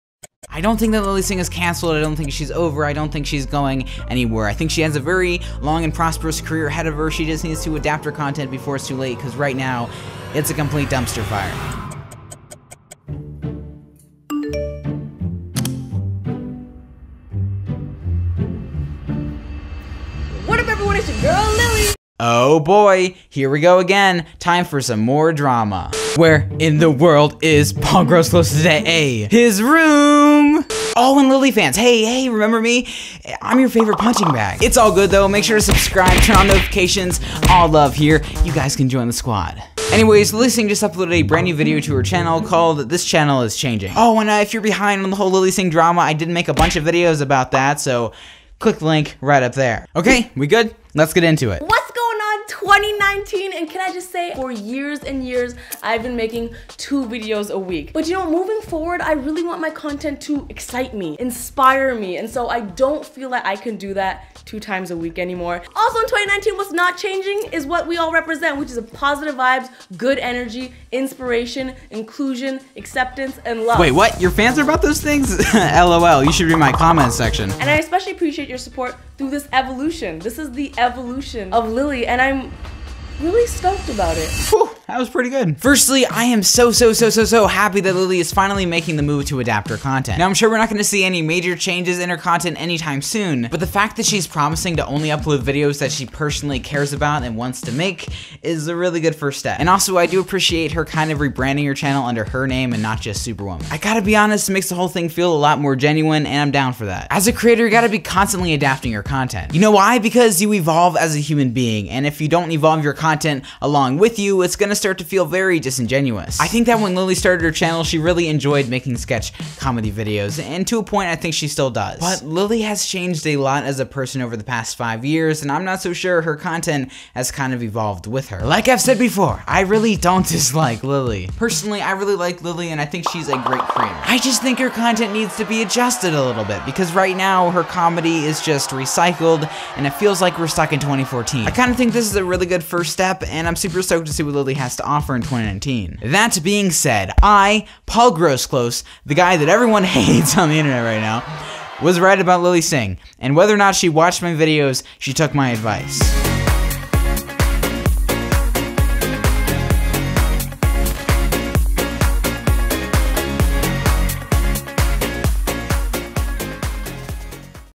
Lilly Singh Took My Advice!!
Lilly Singh (iisuperwomanii) has gone through her fair share of hate and controversies and recently uploaded a video announcing a change regarding her content, that resembled advice I gave her in a past video Lilly Singh's channel is changing, and she took my advice, whether or not she actually saw my video.

Lilly Singh's Channel Is Changing, and SHE TOOK MY ADVICE!! (Does She Hate Me?)

-M E T A-

At the time of the release, the channel holds 2.9K subscribers

If you actually read the description, what are your thoughts on Lilly Singh's career?